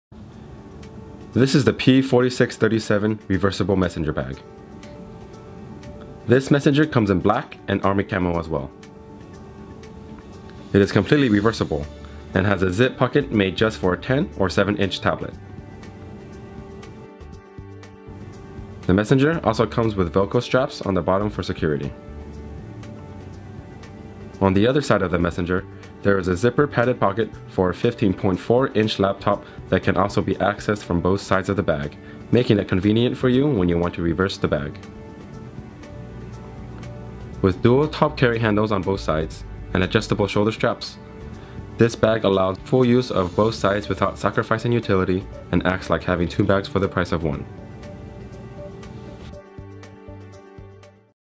P4637 Reversible Messenger
Made of 600D rip-stop polyester, this highly innovative messenger is completely reversible, with pockets for all your needs. The sleek messenger just requires one flip to double the duty of the bag! One side features a padded tablet sleeve to fit both 10" and 7" tablets, a media pocket, a phone pocket, and a pocket for incidentals. The other side of the flap features a roomy zip compartment for your files, documents, and stationery. The back flap boasts a padded, zippered 15.4" laptop compartment with zippers on both sides to allow universal access to the laptop compartment. Dual top carry handles on both sides, and removable shoulder straps, allow full use of both sides without sacrificing utility. It's like having two bags for the price of one!